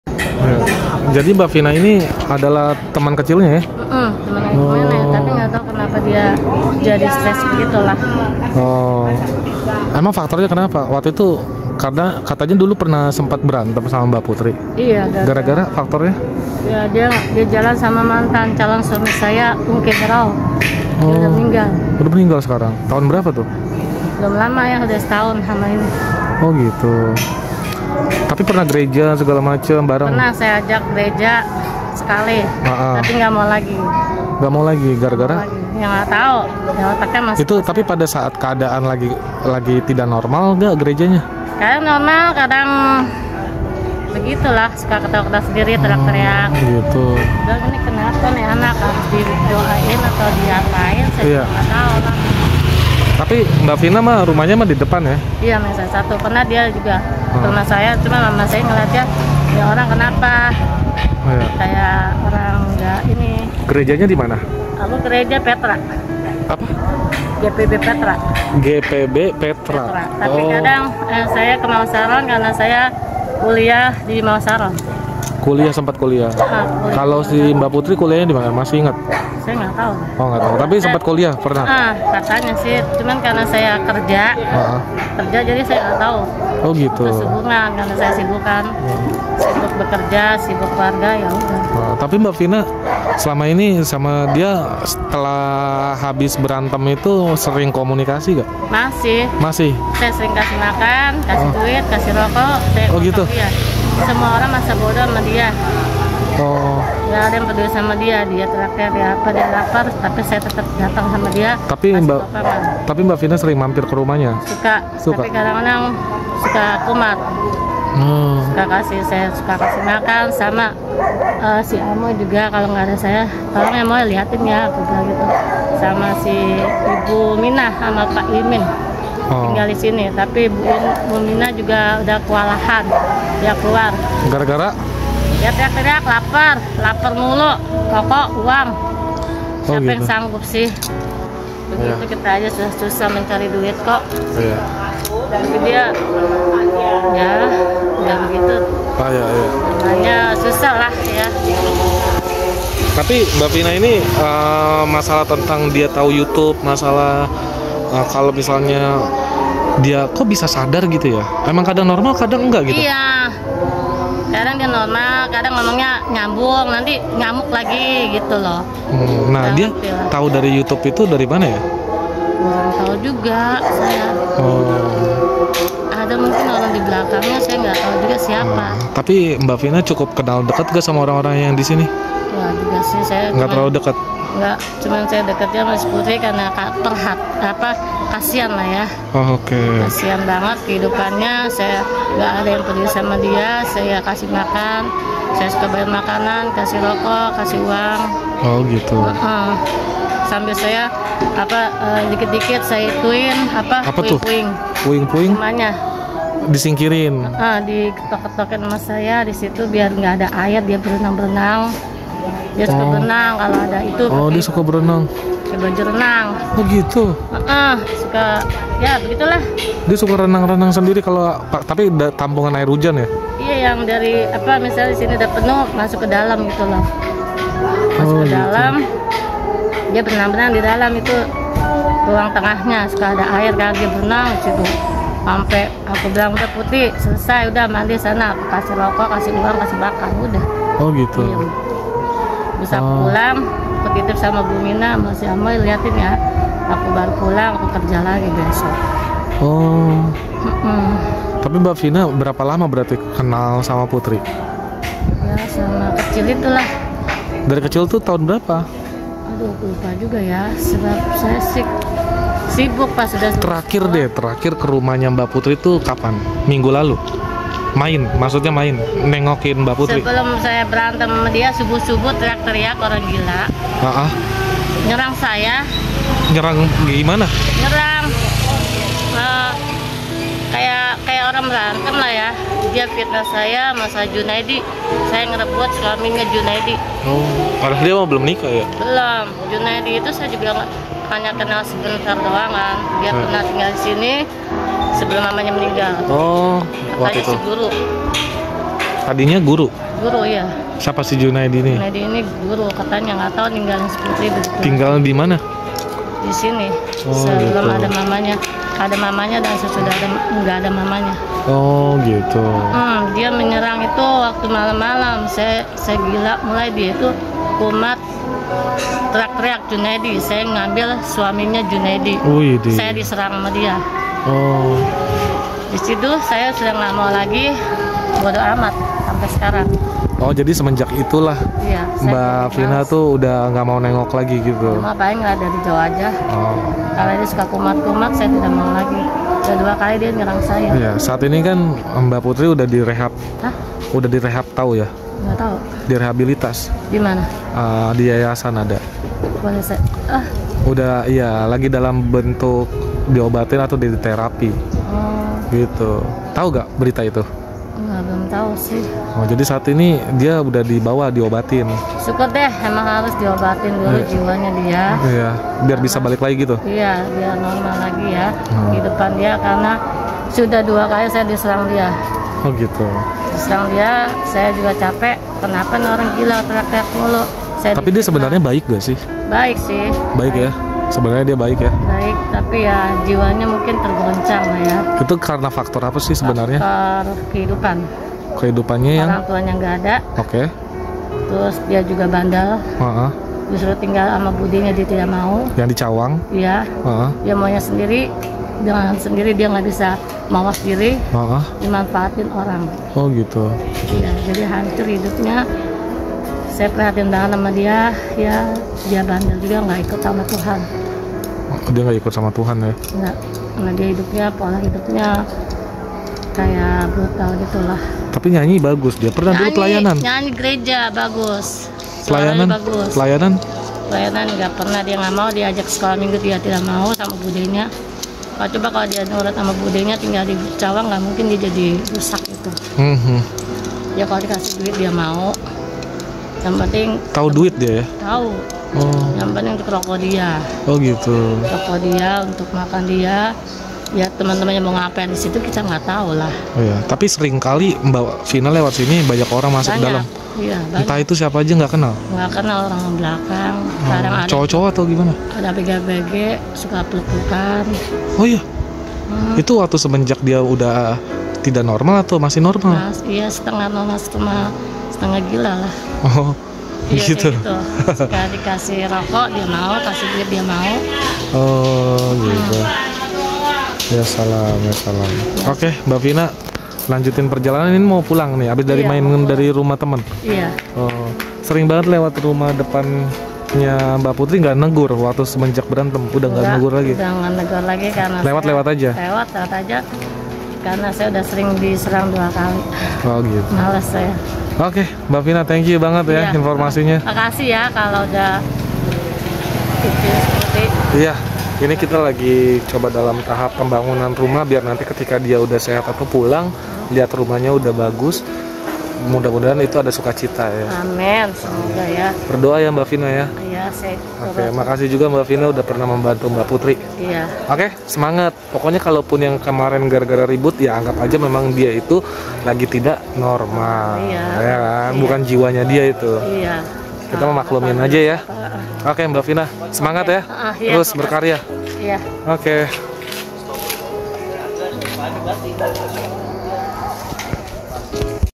INILAH SOSOK SAHABAT MBA PUTRI YANG KINI TELAH HADIR!
Mba vina adalah sosok sahabat mba putri sejak kecil,
gereja bareng, main bareng seperti sudara.
pernah juga selisih dikarenakan pria.
Yahhh, gitu deh..

pokoknya smlekentep,
Salam Toleransi,
GBU

ID BIGO:
Baks888

ID TIKTOK:
baks_888

NOICE:
dibalikMalam